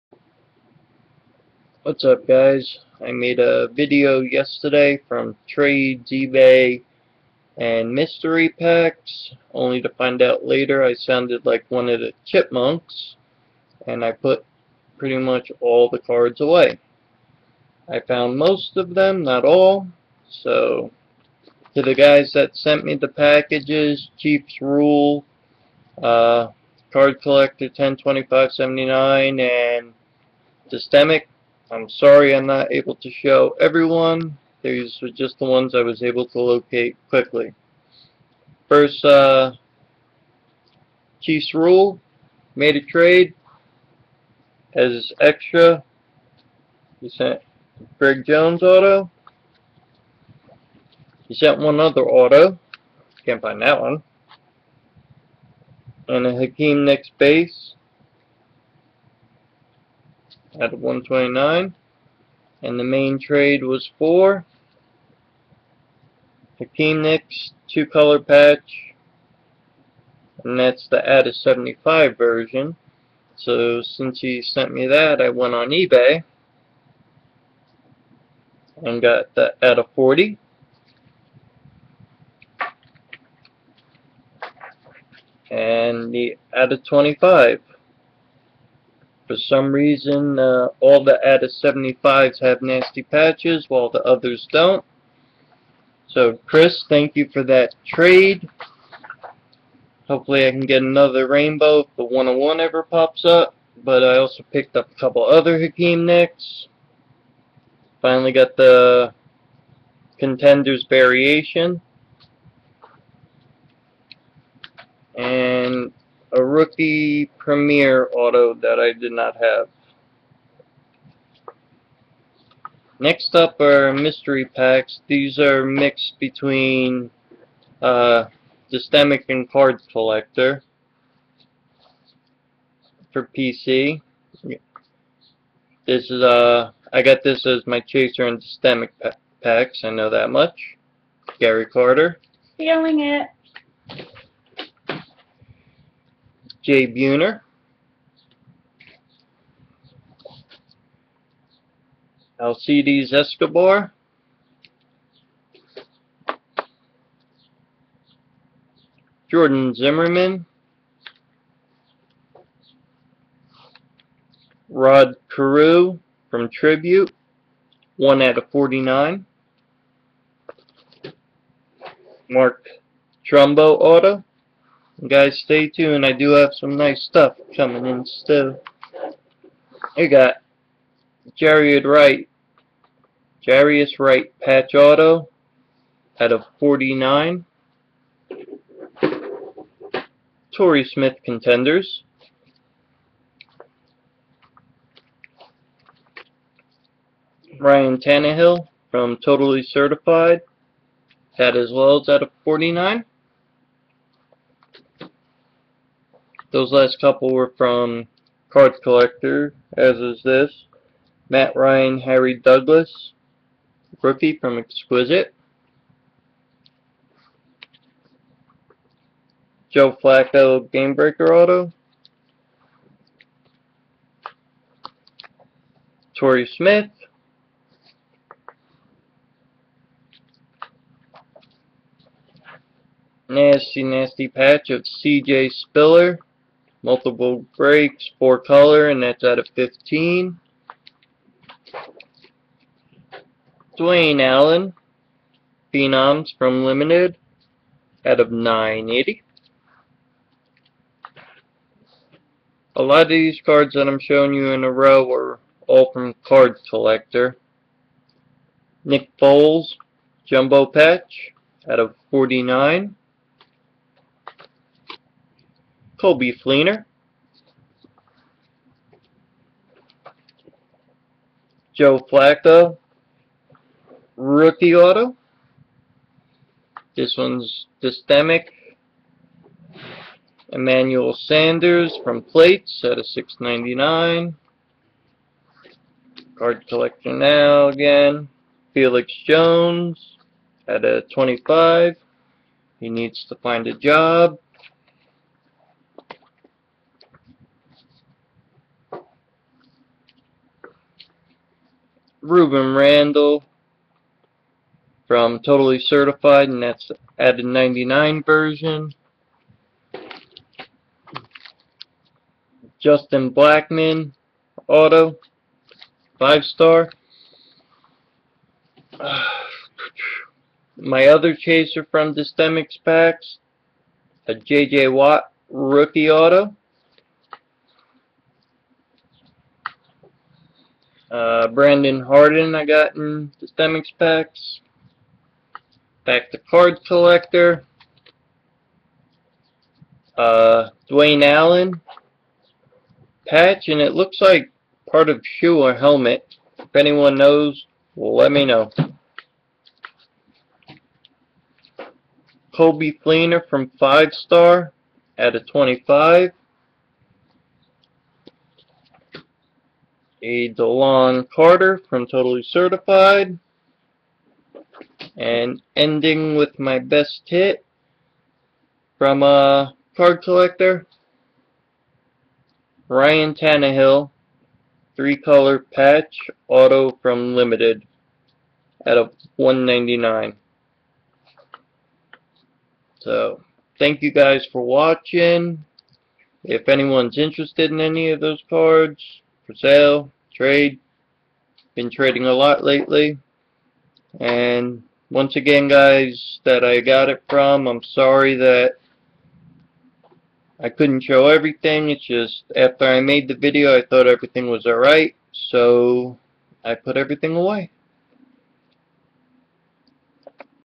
Ebay Trade and Mystery Pack Mail
Nice mailday from chiefsrules, distemic and cardcollector102579